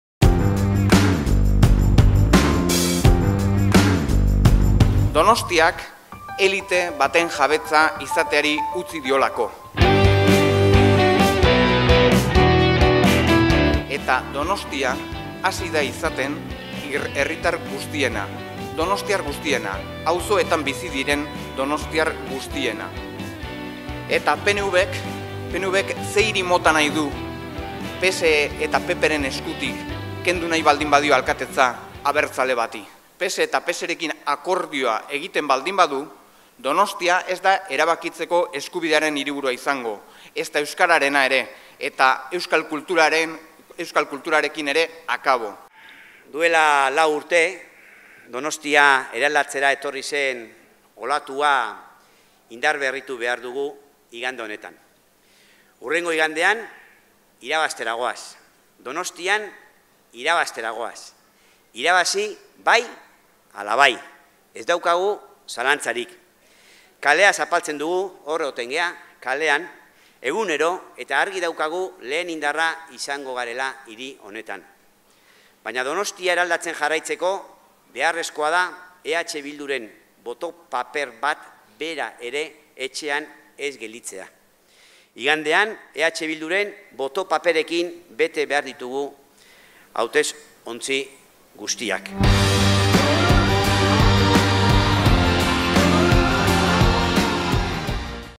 Izagirre: "Kalea zapaltzen dugu egunero eta argi daukagu lehen indarra izango garela Donostian"
Donostian egindako ekitaldiaren laburpena